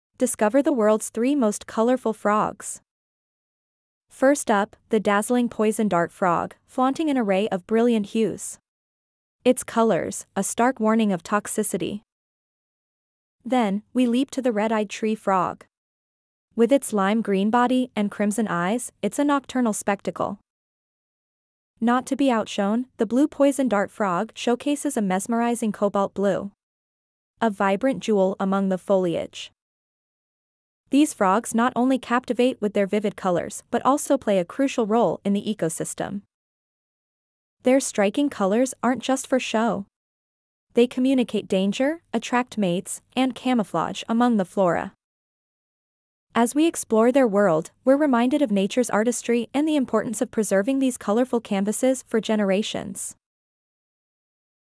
Three Most Colorful Frogs in the World
Step into the enchanting world of amphibians and behold the splendor of nature's palette with the three most colorful frogs on the planet. In this captivating video, discover the mesmerizing hues and intricate patterns of these tiny creatures as they inhabit their natural habitats across the globe.